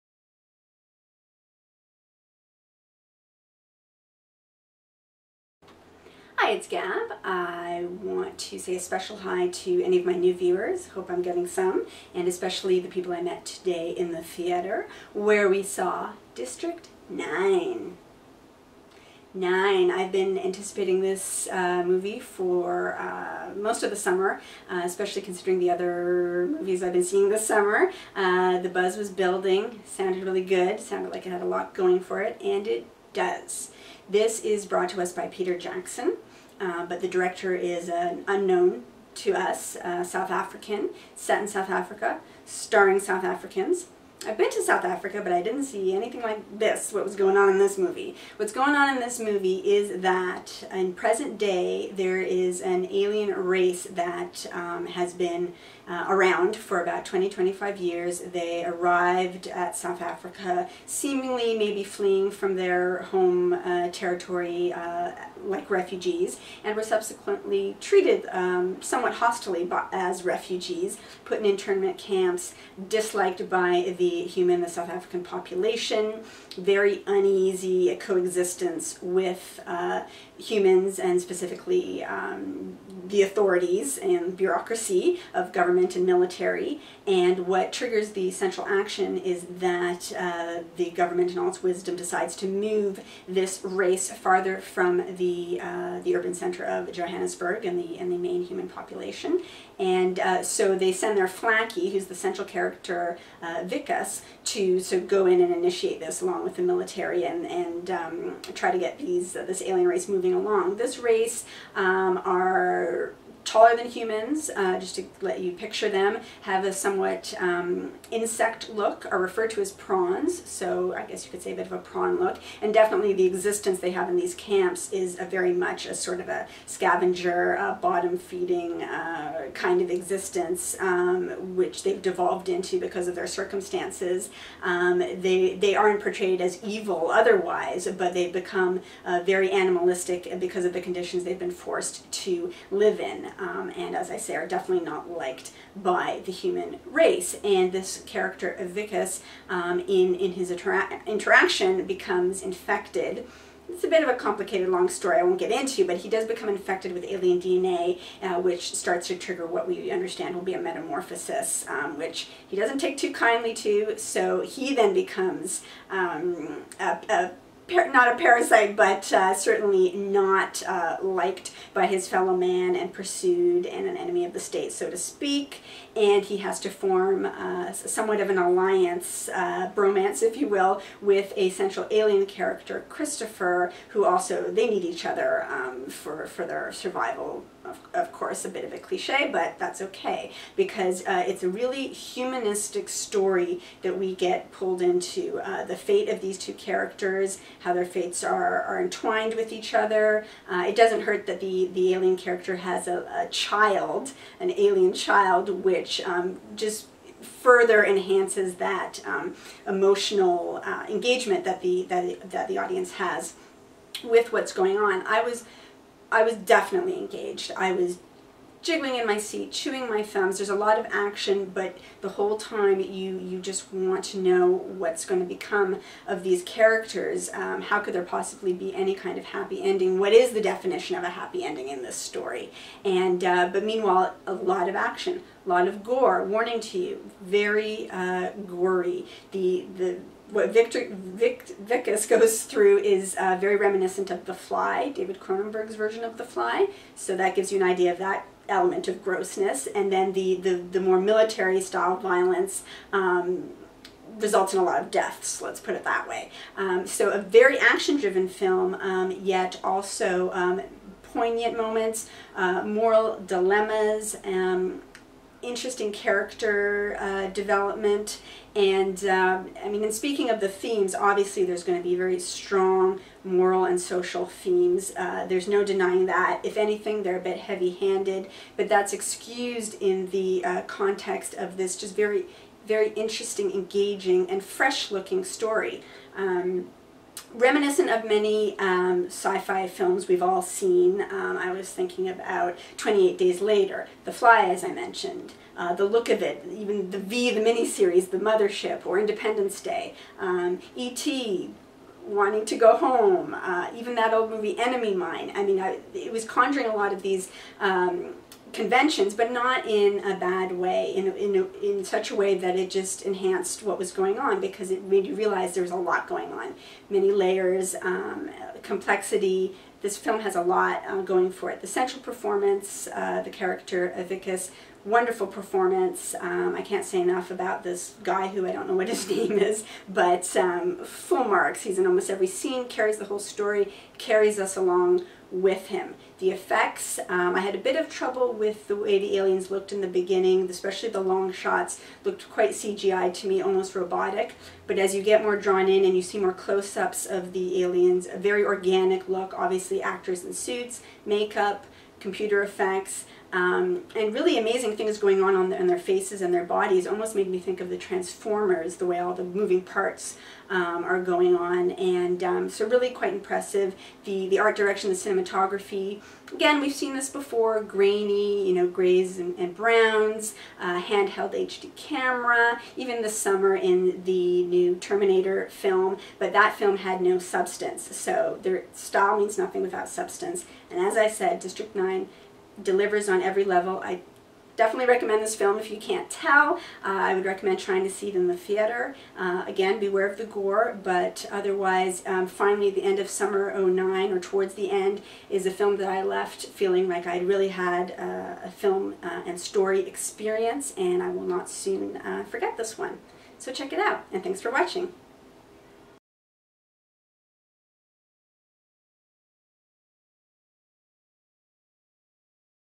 Gab reviews District 9
Gab's review for this latest DVD release - based on its theatrical presentation, with added info bites about the DVD features. For humans only!